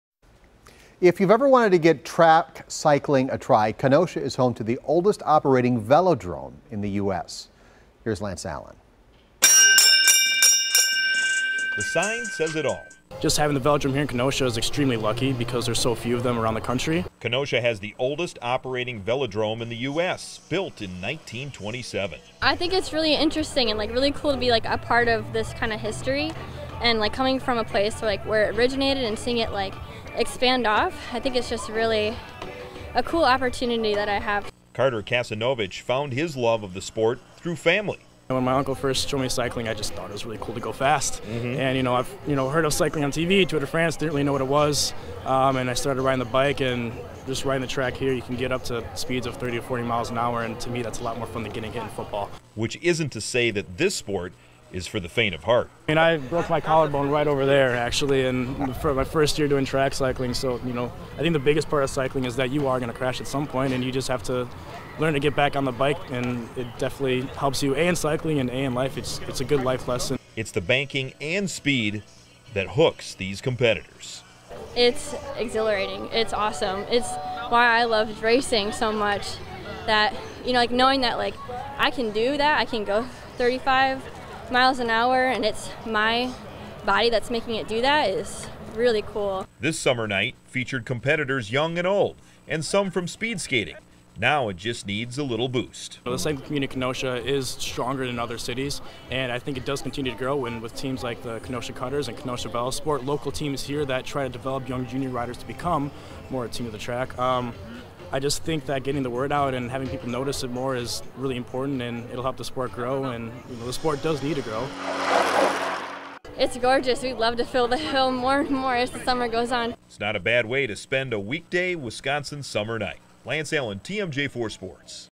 Kenosha has the oldest operating velodrome in the U.S.
Kenosha has the oldest operating velodrome in the U.S., built in 1927.